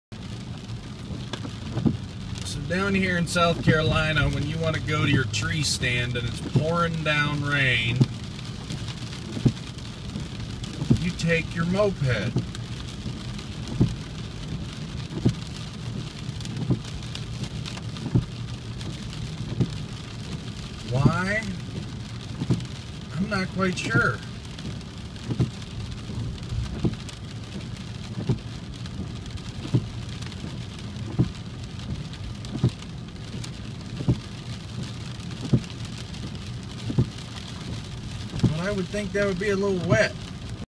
Moped in the rain
In SC people ride these mopeds everywhere no matter what the weather. I have no idea why.